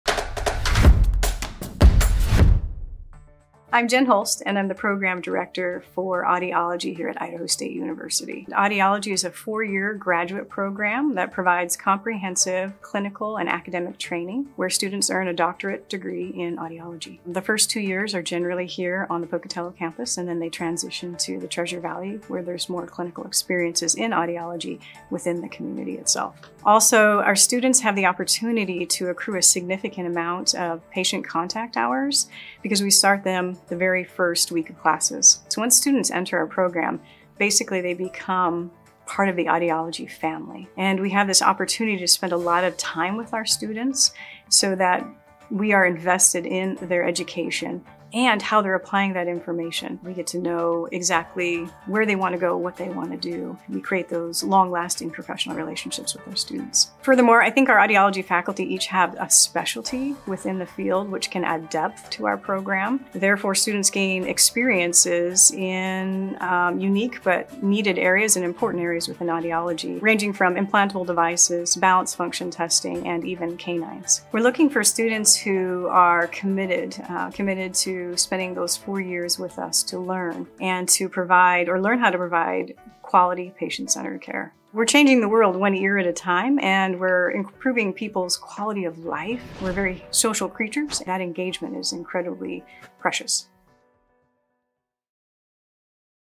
Audiology Program - Idaho State University
To learn more about Idaho State's Audiology program visit their

The Audiology program, within the School of Communication Science and Disorders, offers coursework at the undergraduate and graduate levels.